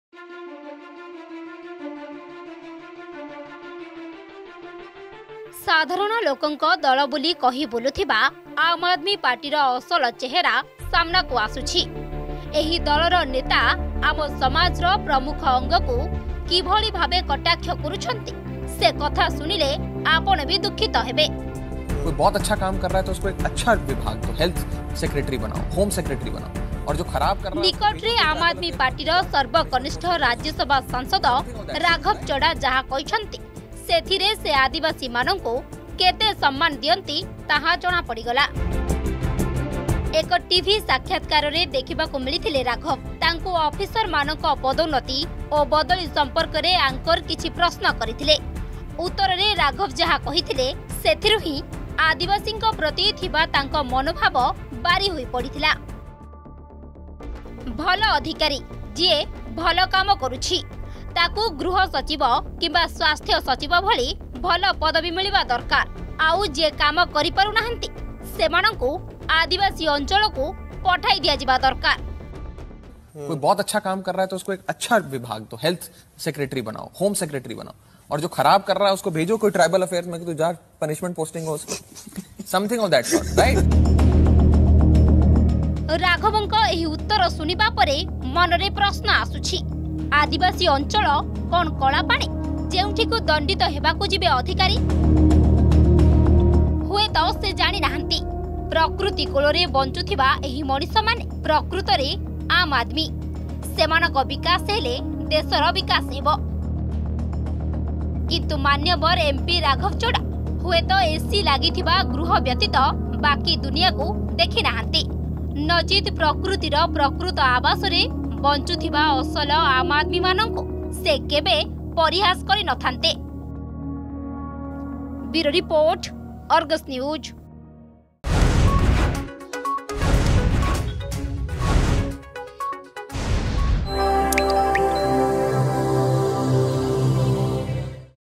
ଆମ ଆଦମୀ ଦଳ, ଆମ ଆଦମୀକୁ ଏମିତି ସମ୍ମାନ ଦିଏ
ଏକ ଟିଭି ସାକ୍ଷାତକାରରେ ଦେଖିବାକୁ ମିଳିଥିଲେ ରାଘବ। ତାଙ୍କୁ ଅଫିସରମାନଙ୍କ ପଦୋନ୍ନତୀ ଓ ବଦଳି ସମ୍ପର୍କରେ ଆଙ୍କର କିଛି ପ୍ରଶ୍ନ କରିଥିଲେ। ଉତ୍ତରରେ ରାଘବ ଯାହା କହିଥିଲେ, ସେଇଥିରୁ ହିଁ ଆଦିବାସୀଙ୍କ ପ୍ରତି ଥିବା ତାଙ୍କ ମନୋଭାବା ବାରି ହୋଇପଡିଥିଲା।